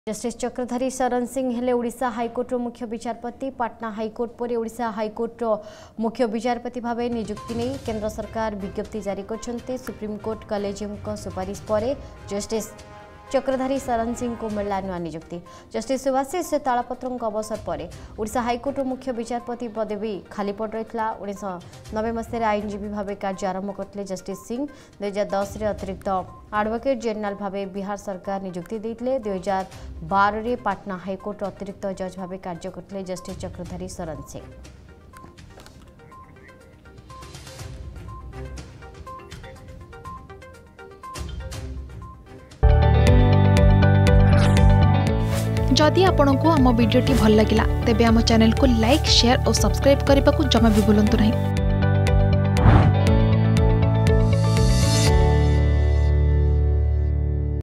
ଓଡ଼ିଶା ହାଇକୋର୍ଟର ମୁଖ୍ୟ ବିଚାରପତି ହେଲେ ଜଷ୍ଟିସ୍ ଚକ୍ରଧାରୀ || Odisha High Court New Chief Justice
ଜଷ୍ଟିସ୍ ଚକ୍ରଧାରୀ ଶରନ୍ ସିଂହ ହେଲେ ଓଡ଼ିଶା ହାଇକୋର୍ଟର ମୁଖ୍ୟ ବିଚାରପତି। ପାଟନା ହାଇକୋର୍ଟ ପରେ ଓଡ଼ିଶା ହାଇକୋର୍ଟର ମୁଖ୍ୟ ବିଚାରପତି ଭାବେ ନିଯୁକ୍ତି ନେଇ କେନ୍ଦ୍ର ସରକାର ବିଜ୍ଞପ୍ତି ଜାରି କରିଛନ୍ତି ।